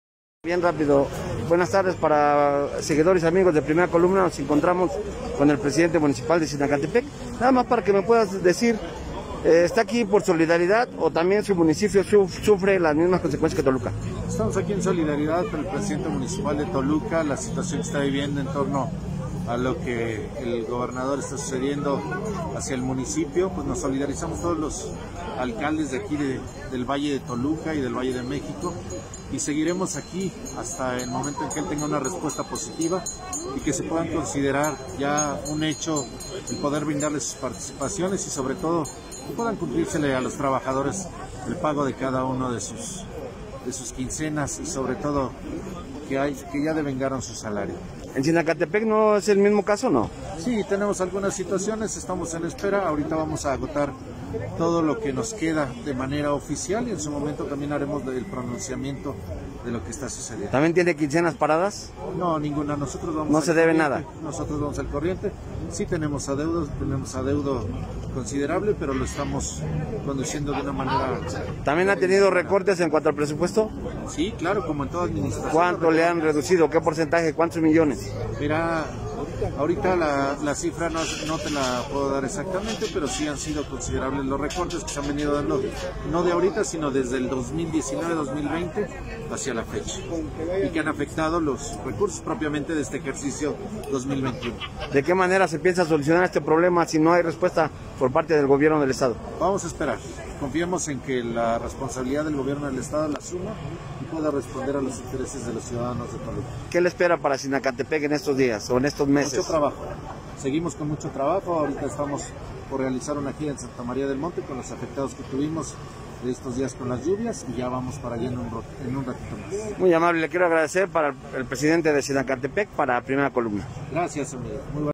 Primera Columna Mx | Somos presidentes solidarios: Gustavo Vargas presidente de Zinacantepec
Toluca, Estado de México. "En solidaridad con el alcalde de Toluca decidimos unirnos a las demandas de los recursos para los ayuntamientos": Gustavo Vargas presidente municipal de Zinacantepec.